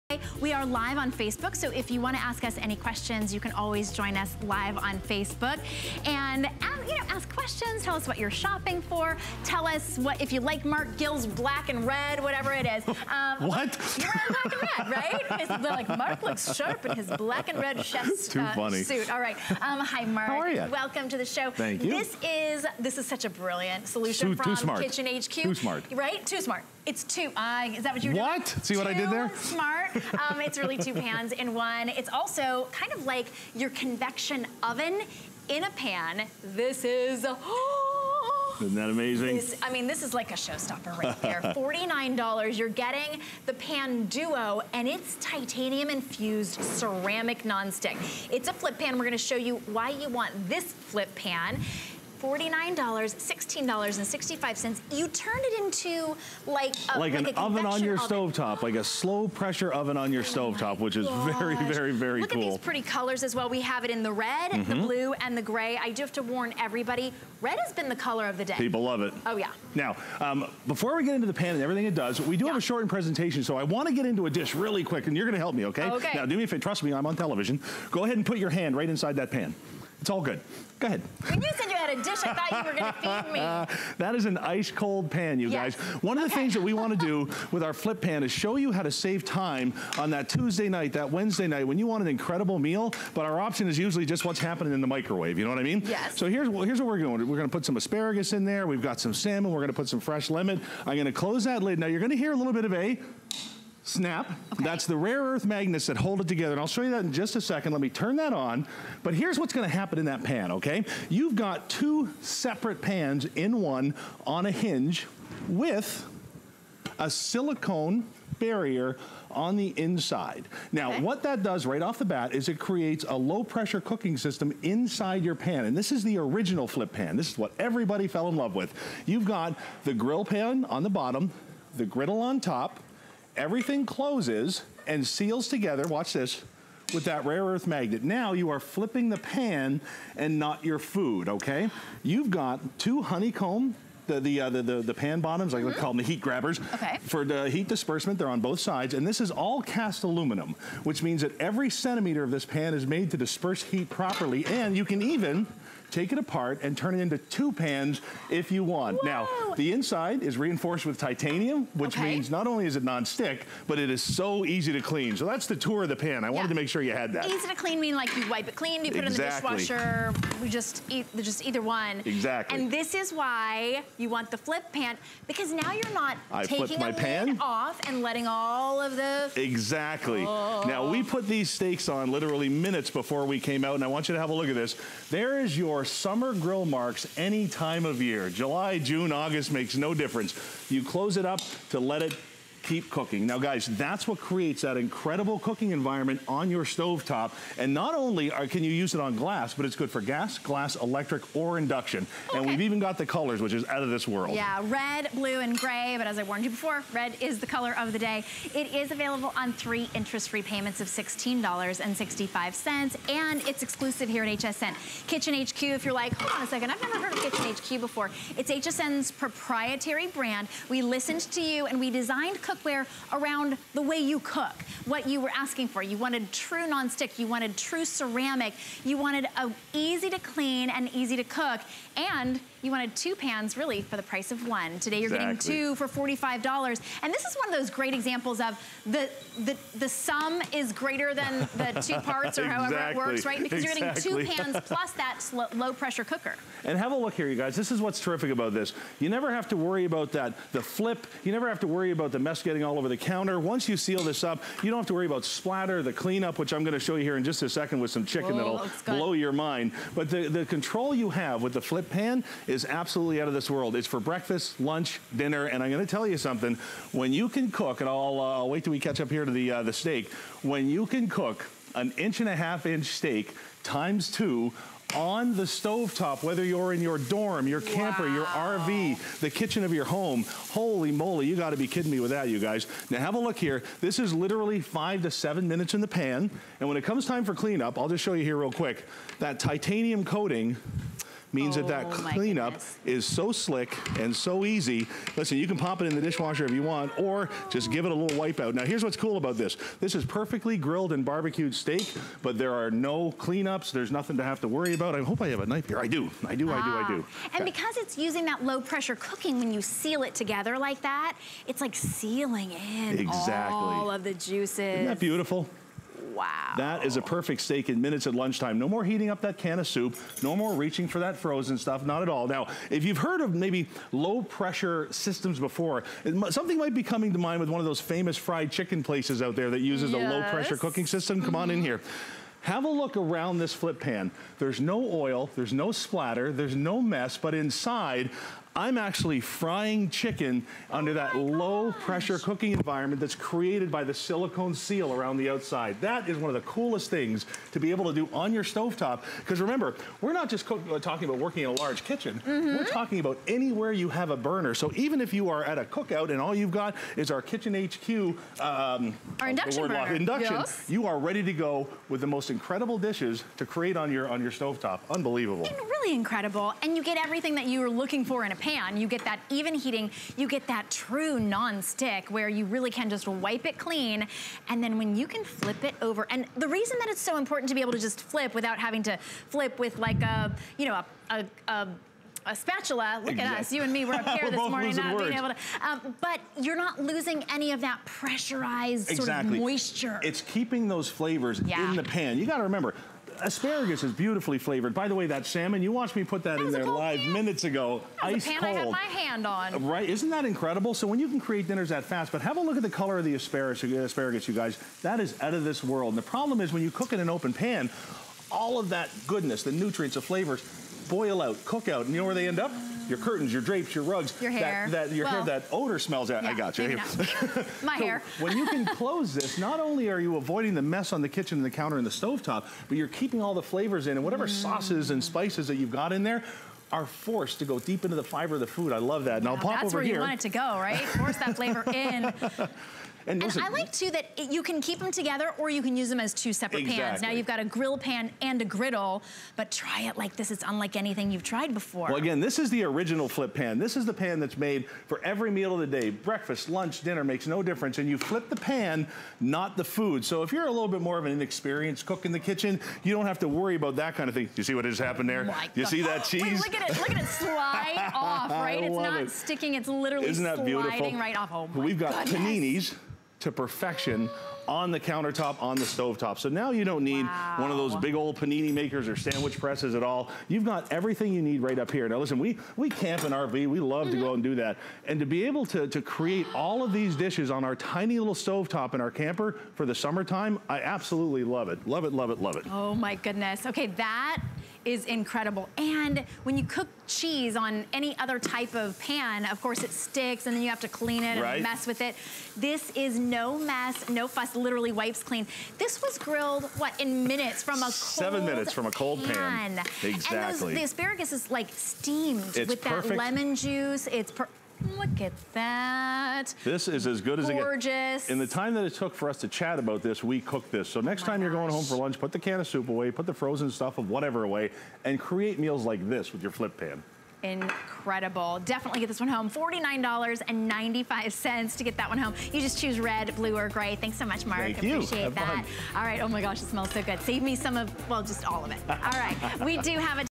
Kitchen HQ TitaniumInfused Ceramic Nonstick Flip Pan
Separate the Flip Pan into two separate pans, or use them to make cooking and flipping certain foods  like omelets, pancakes and fish fillets  way easier. The nonstick interior coating is infused with titanium, adding to its durability and enhancing its releasing capabilities.

    Top griddle pan
    Lower base grill pan